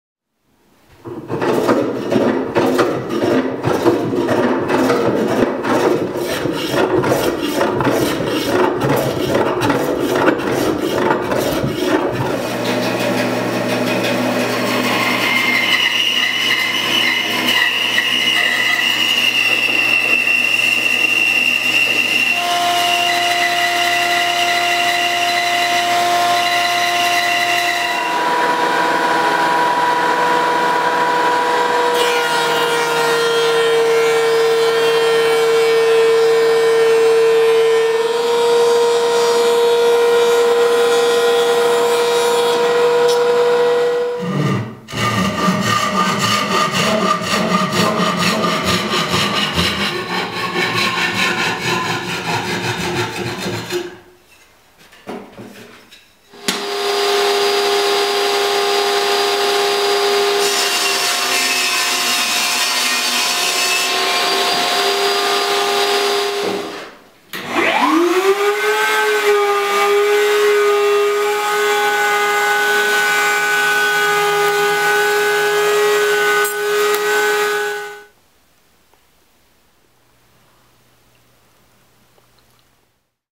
Making Skunk Strips for Electric Guitar Necks
Not a tutorial but a film to display the methods i use to creating these essential walnut strips for one piece electric guitar necks. Many more videos and tutorials to come.